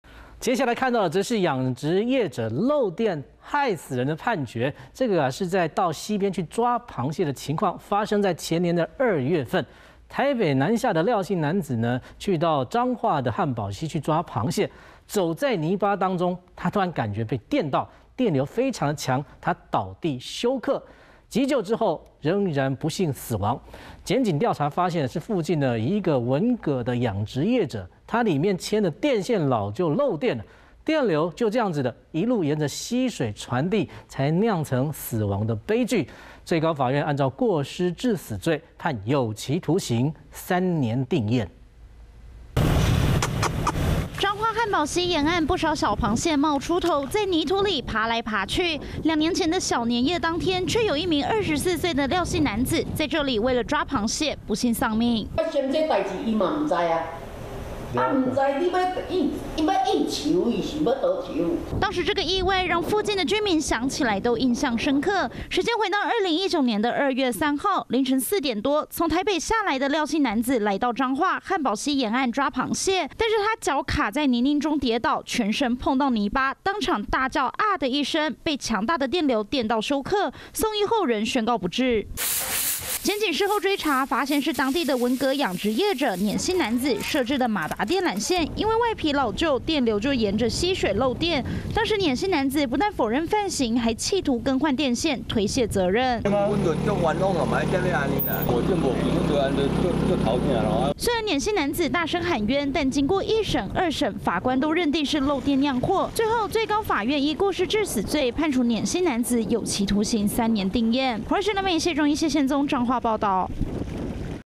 男抓螃蟹遭電死 漏電養殖業者被判刑｜華視新聞 20210815
到溪邊抓螃蟹，卻慘遭電死，這是發生在前年二月份，小年夜當天凌晨，從台北下來的廖姓男子，和朋友到彰化漢寶溪抓螃蟹，當時走在泥濘中的他，突然被電到，倒地休克，經過急救仍不治身亡，檢警調查，發現是附近的粘姓文蛤養殖業者，設置的電線老舊漏電，電流一路沿著溪水傳遞，才釀成悲劇，最高法院依過失致死罪，判有期徒刑3年定讞。